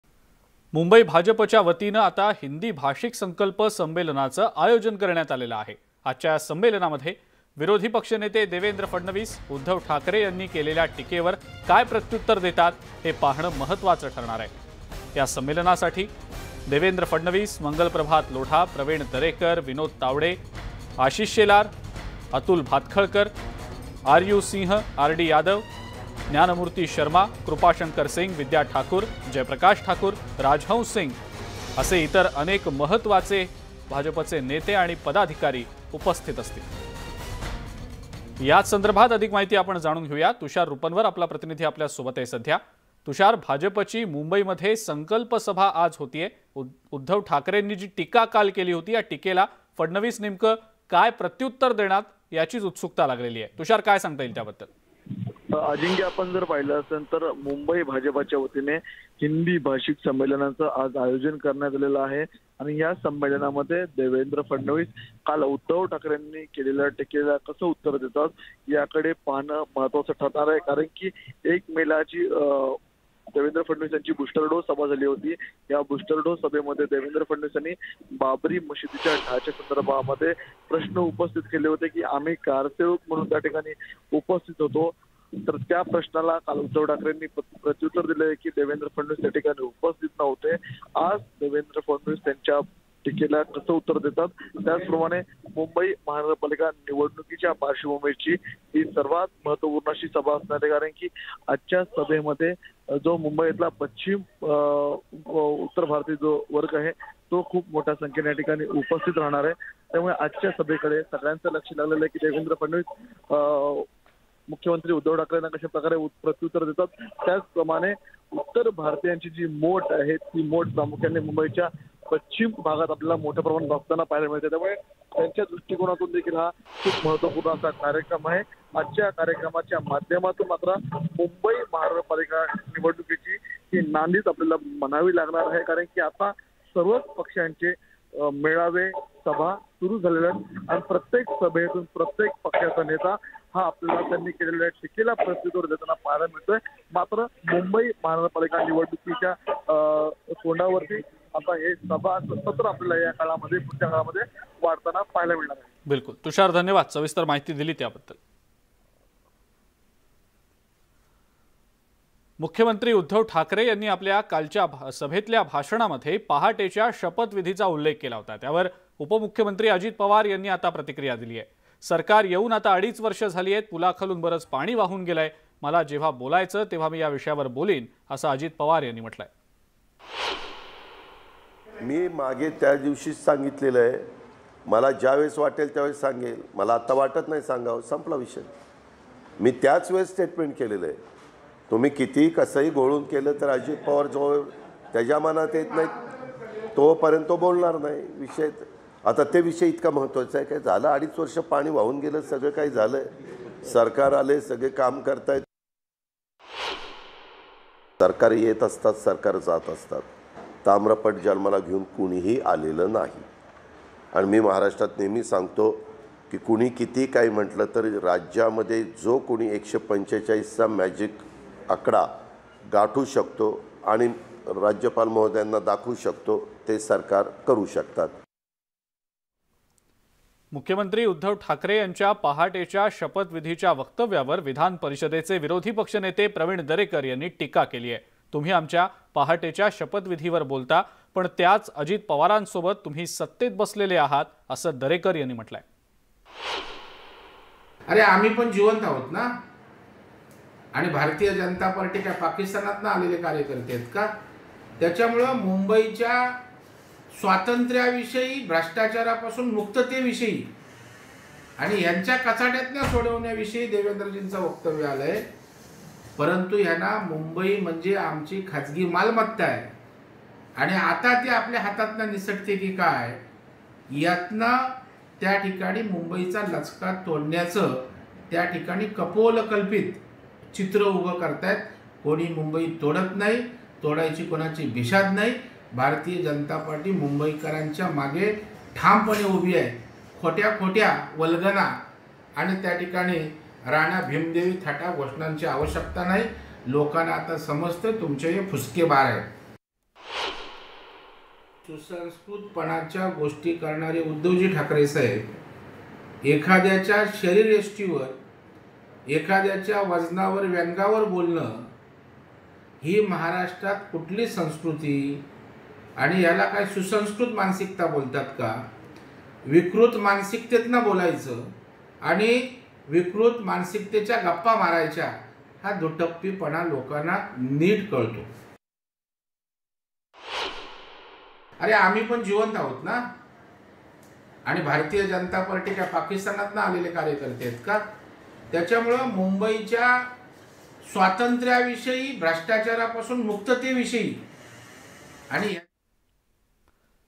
Devendra Fadnavis Sabha : फडणवीस आज 'ठोक के जवाब' देणार
शिवसेना पक्षप्रमुख आणि राज्याचे मुख्यमंत्री उद्धव ठाकरे (CM Uddhav Thackeray) यांची 14 मे रोजी सभा पार पडली. त्यानंतर लगेच दुसऱ्याच दिवशी म्हणजे 15 मे रोजी देवेंद्र फडणवीस यांची सभा होणार आहे. देवेंद्र फडणवीस यांची याआधी 1 मे महाराष्ट्र दिनाचे औचित्य साधत मुंबईच्या सौमय्या मैदानावर सभा झाली होती. त्यावेळी फडणवीसांनी शिवसेनेवर (Shiv Sena) सडकून टीका केली होती. आता मुख्यमंत्री ठाकरे यांच्या टीकेनंतर फडणवीस काय बोलणार याकडे राज्याचं लक्ष लागलं आहे.